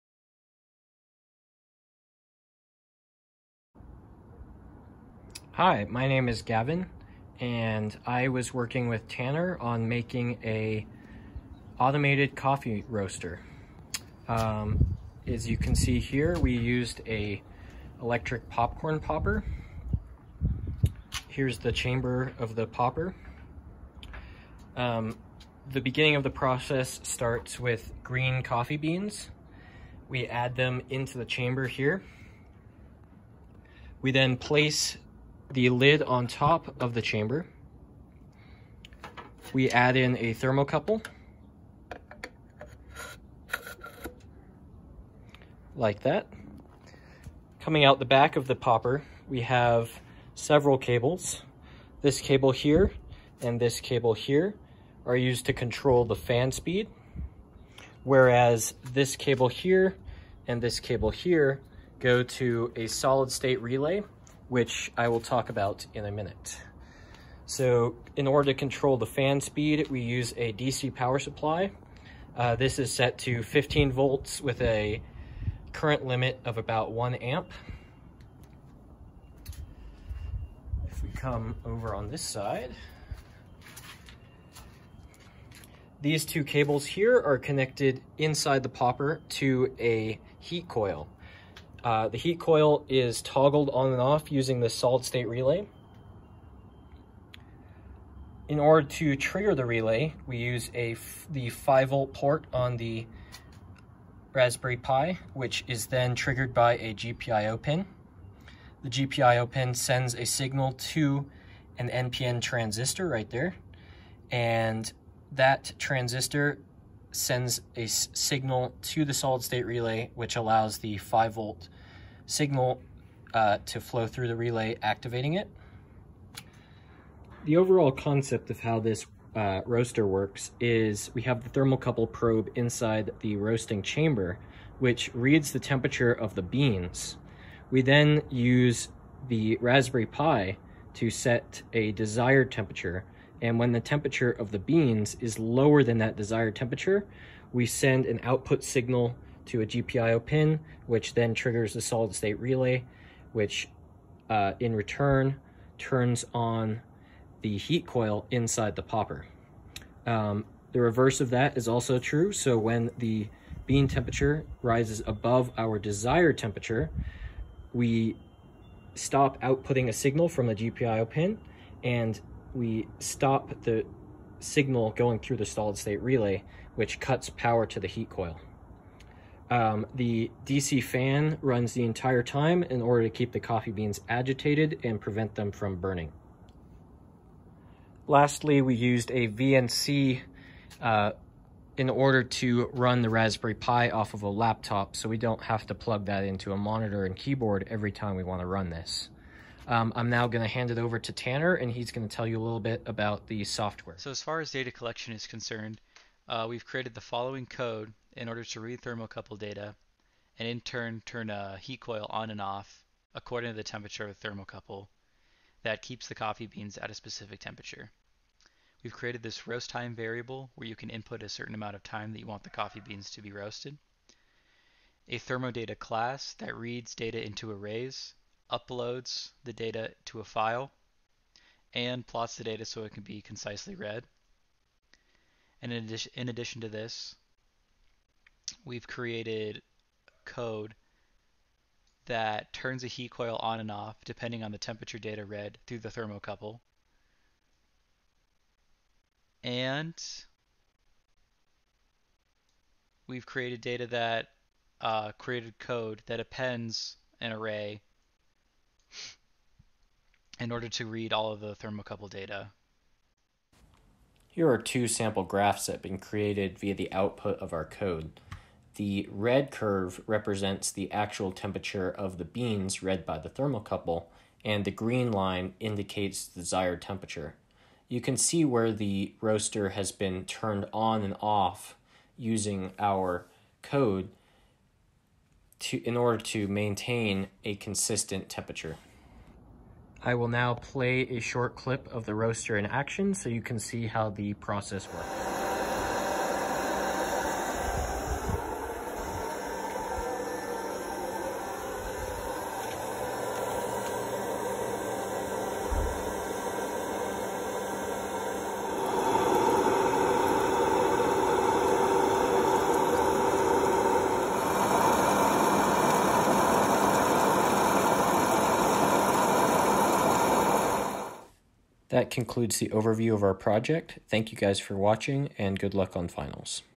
CSCI 250 Capstone Project: Digital Coffee Roaster
Video presentation of our final Capstone project.